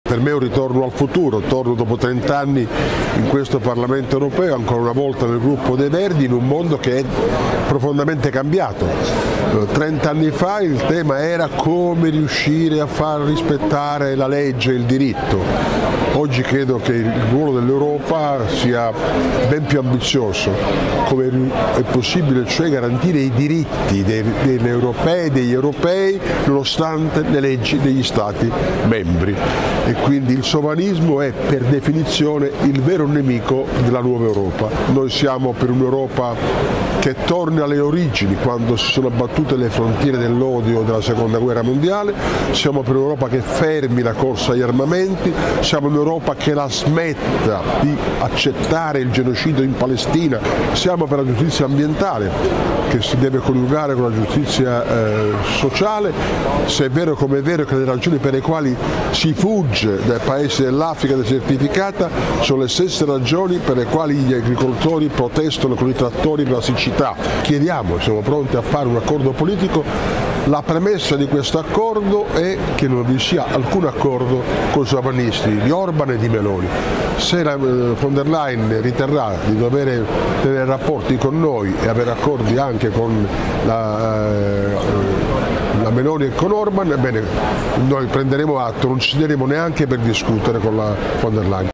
Insediamento dell'Eurocamera, Orlando: "Il sovranismo è il vero nemico della nuova Europa"
Estratto dell'intervista a Leoluca Orlando, eurodeputato di Alleanza Verdi e Sinistra (nel gruppo dei Verdi europei), a Strasburgo per la sessione plenaria d'insediamento del Parlamento europeo.

16/07/24 - di Simone De La Feld